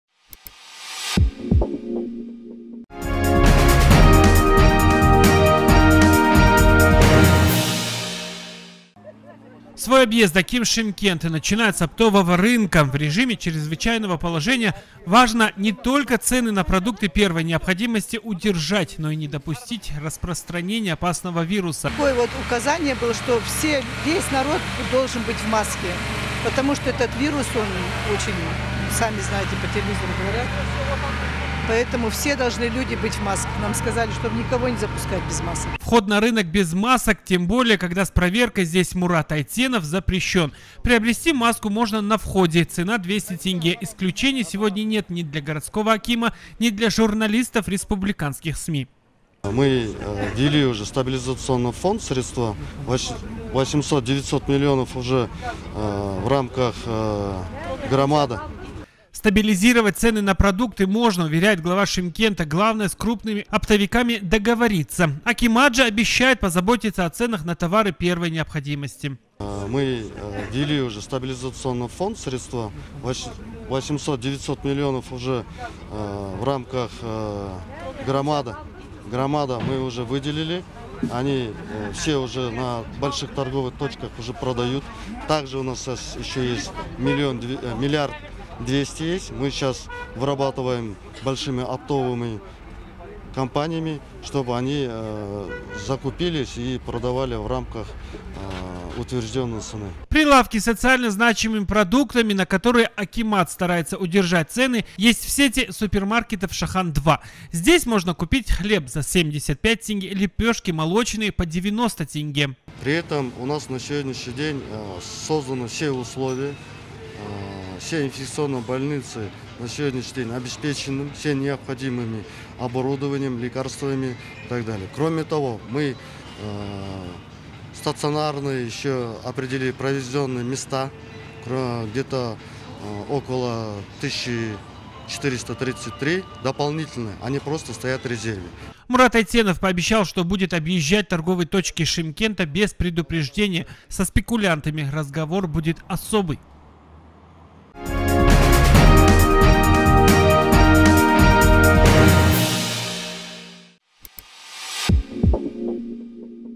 Как в Шымкенте будут сглаживать скачки цен на продукты /Экстренный выпуск по коронавирусу в Шымкенте
Как в Шымкенте будут сглаживать скачки цен на продукты // Глава третьего мегаполиса во время объезда оптовых рынков и супермаркетов сделал несколько замечаний хозяевам. В частности, в общественном месте все должны ходить в масках, нужно соблюдать гигиену, — призвал Айтенов.
Аким планирует посредством стабилизационного фонда сдерживать цены на местном рынке, средства выделены, ведутся переговоры с крупными оптовыми компаниями.